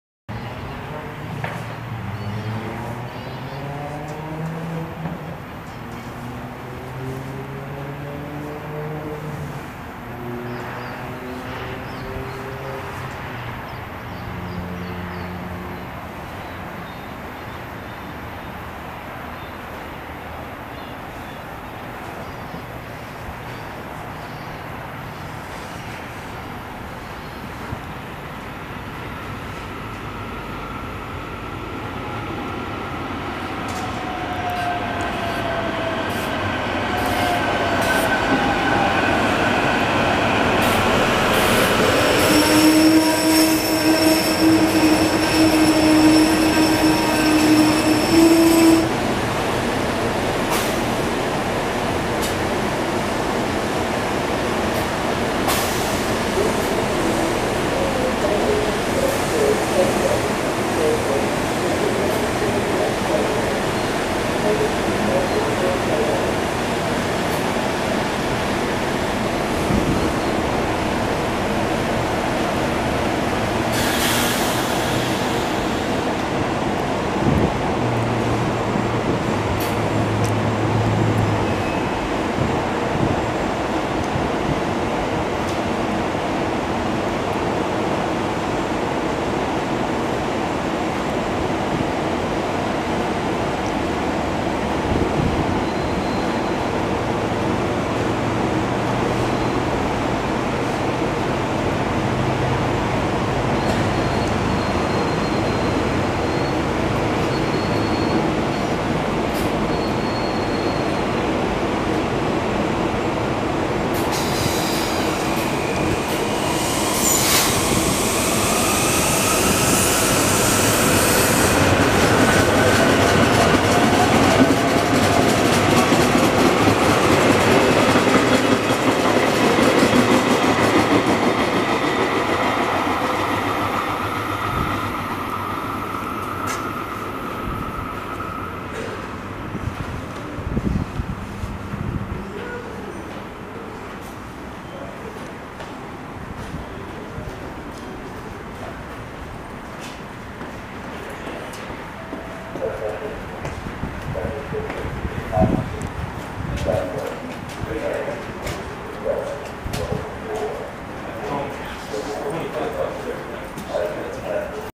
SEPTA Regional Rail Full HD: Trenton Line Train 765 at Levittown, PA
Shown entering & leaving the station at Levittown, Pennsylvania is SEPTA Regional Rail Trenton Line Train 765 with Silverliner V's bound for Trenton, NJ across the river shortly upon my arrival for a little bit tonight. Shortly after 765 departs an announcement is heard in the background. Enjoy the sight of new commuter rail equipment in full HD.